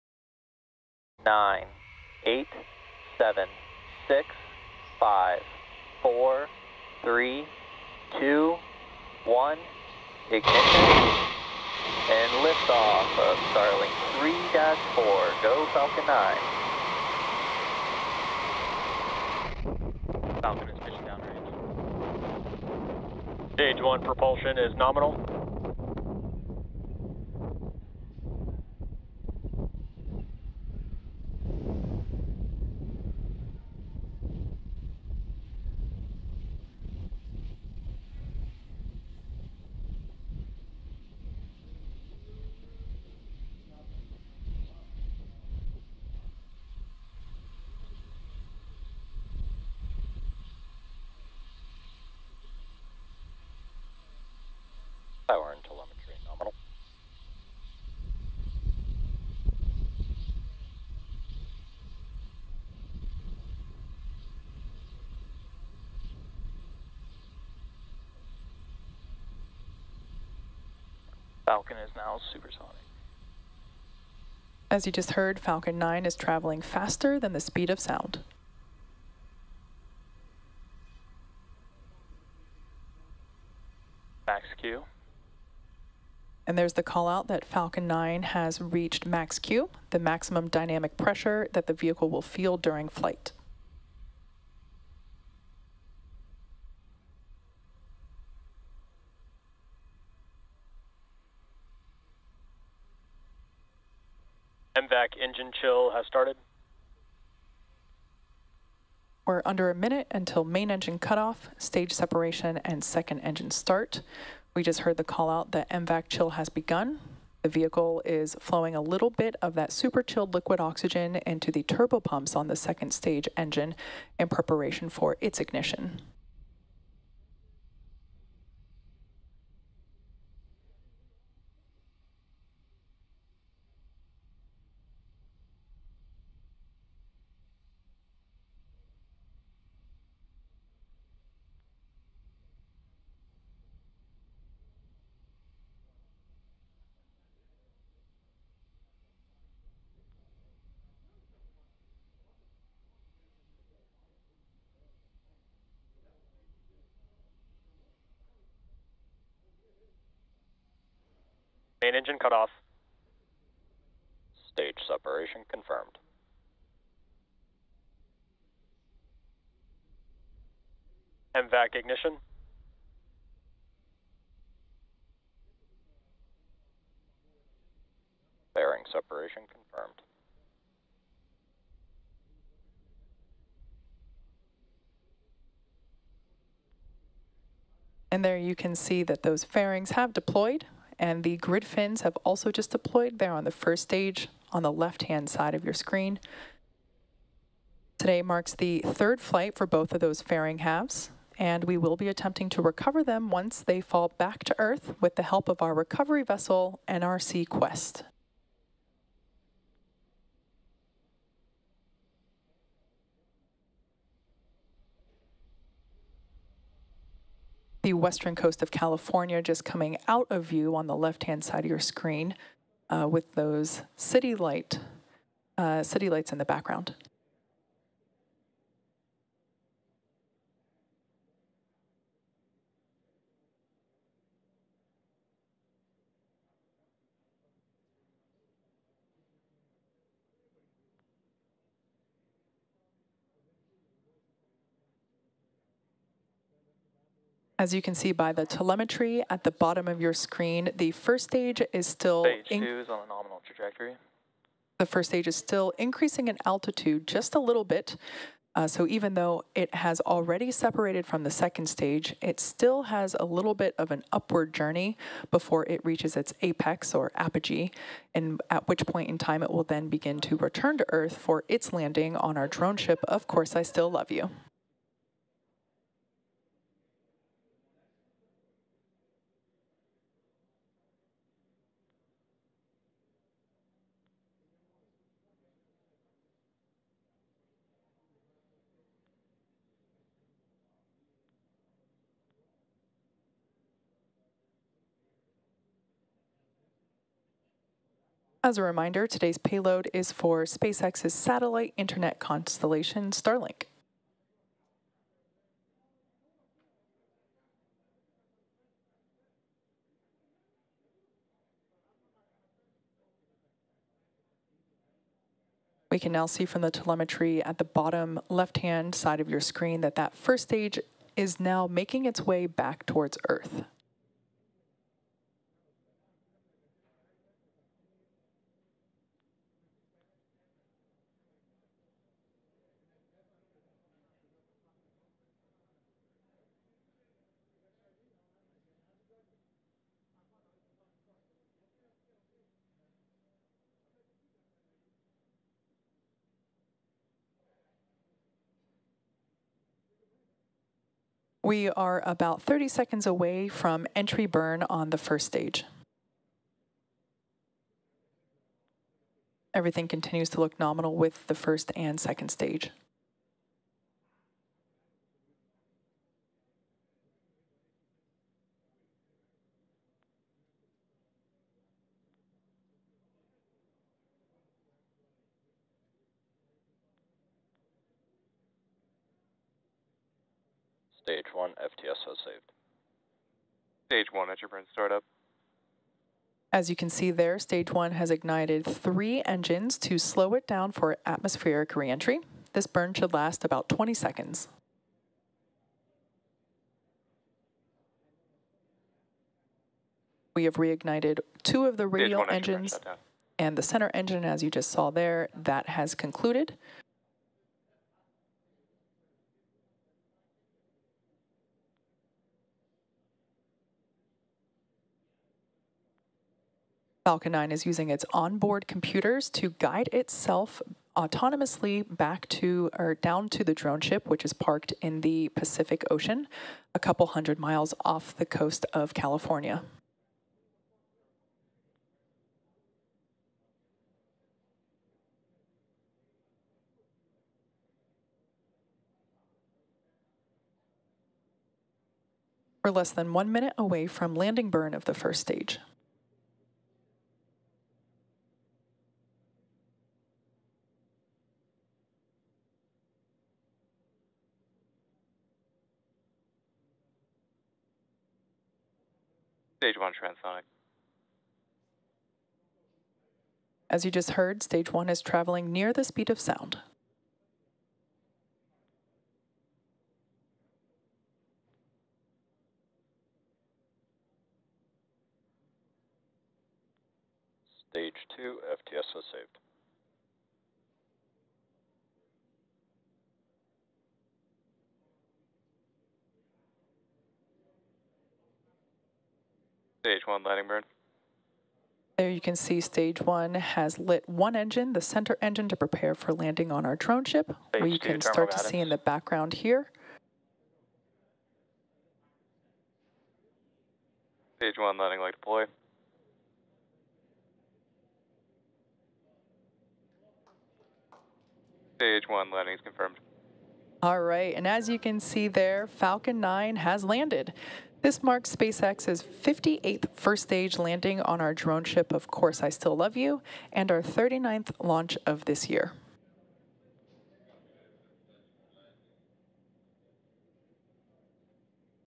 SpaceX launches Starlink batch from Vandenberg, nails landing at sea
A SpaceX Falcon 9 rocket launched 46 new Starlink satellites from Space Launch Complex 4 East (SLC-4E) at Vandenberg Space Force Base in California on July 31, 2022 (July 30, local time). The first stage of the Falcon 9 rocket landed on the "Of Course I Still Love You" drone ship minutes later in the Pacific Ocean. Full Story

Credit: SpaceX